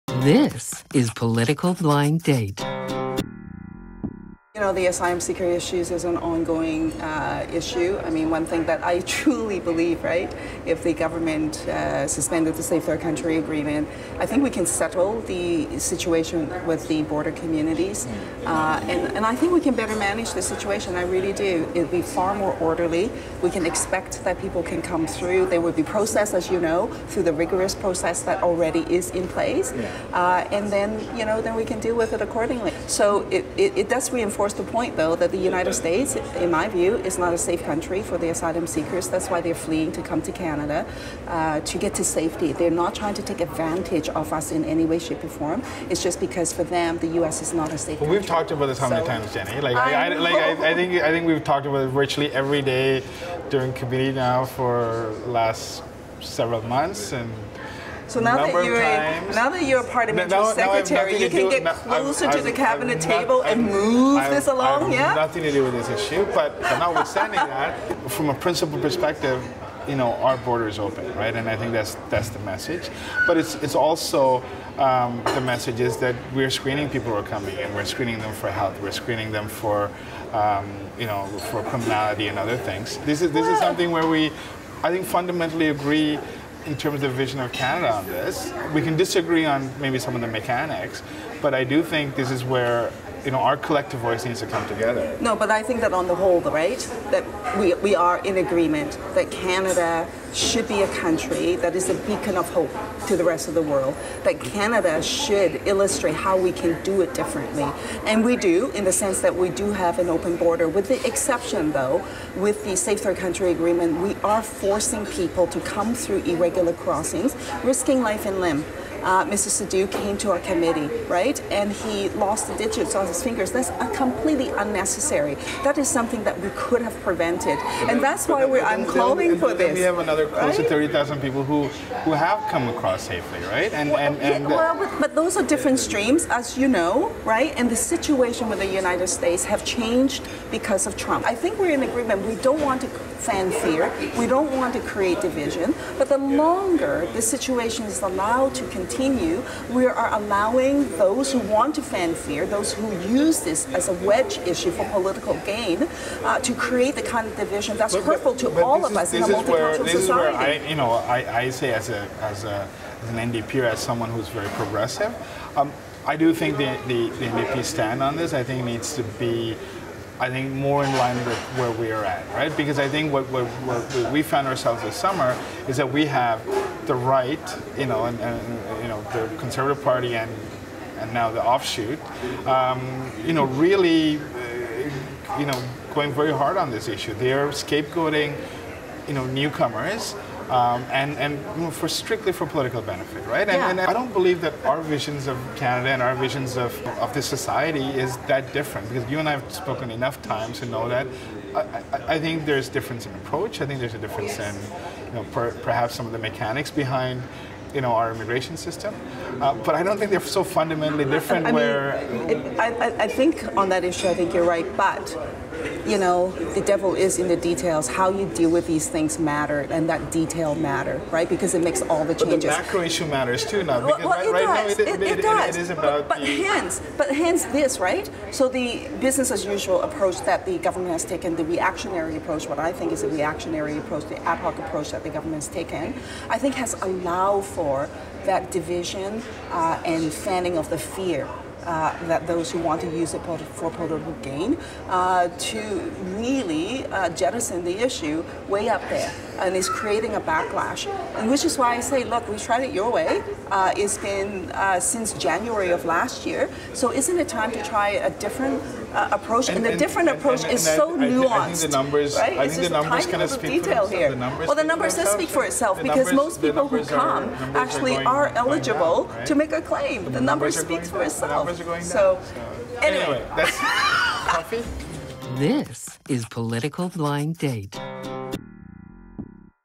Season Two: Episode Five Extra: The Promise of Multiculturalism: US as a 3rd country for refugees
Gary Anandasangaree, Liberal MP and Parliamentary Secretary to the Minister of Multiculturalism, talks with his date, Jenny Kwan, NDP MP and Critic on Immigration, Labour and Multiculturalism, about whether Canada should treat the United States as a non-safe third country in terms of refugees crossing the border .